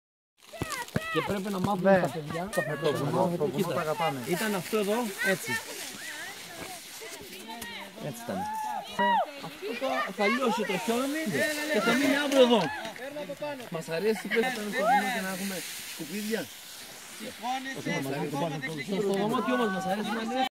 Εκπαιδευση Σκί-Λιβαδιώτικα όρη-Pierra Creta 2017
Pierra Creta 2017...!
Ο μάρτιος πλησιάζει κ το Pierra Creta "ζεσταίνει" τις μηχανές του.Εκπαιδευτές απο xιονοδρομικούς κ ορειβατικούς συλλογους μαθαίνουν σκί στα παιδιά  ωστε να λάβουν μέρος στον αγώνα της 5ης μαρτίου.
Οι φορείς των Λιβαδίων υποστηρίζουν τον αγώνα με όλες τις δυνάμεις τους.Ευχαριστούμε τους φίλους εκπαιδευτές για την υπομονή τους. Παρακολουθηστε πλάνα απο την εκπαίδευση :